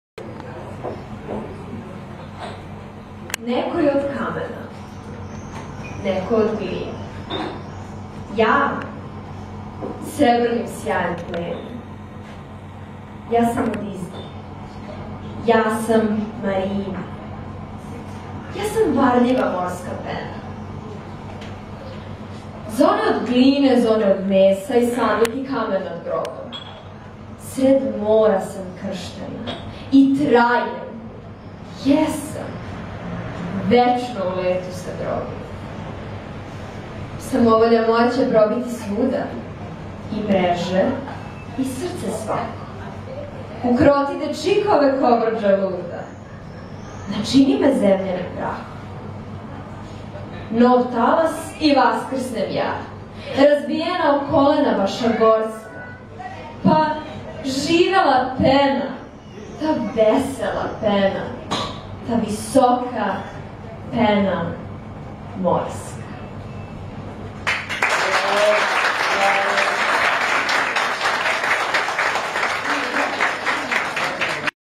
Магично вече 28 - Ања Арнаутовић
Ања Арнаутовић на Магичној вечери 28 поезије, плеса, перформанса, музике, афоризма... Клуб Тулуз Лотрек, Ресавска 24. Београд, 28 јун 2020.